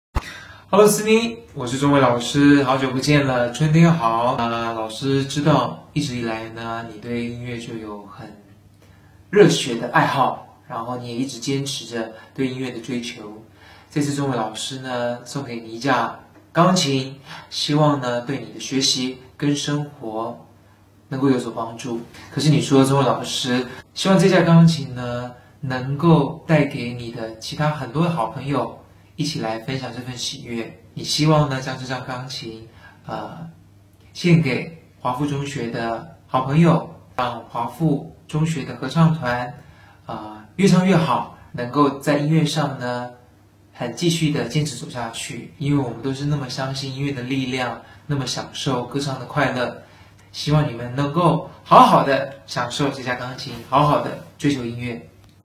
20140222 杨宗纬 华富中学 钢琴捐赠
宗緯把在中國音超獲得的鋼琴贈予中國新聲代愛徒思霓的學校
同学们期盼已久的钢琴下午如期来到我们华富中学了！好开心！我也好高兴能和同学一起感受到@杨宗纬 老师对音乐的挚爱对音乐的传𠄘态度及对我们的关爱！他把自己在金钟奖中国音超获得的歌王奖品-三角钢琴，对他而言这么有意义的奖品送给我们这所普通中学，送给这么多热爱音乐的同学们。大家都好感动！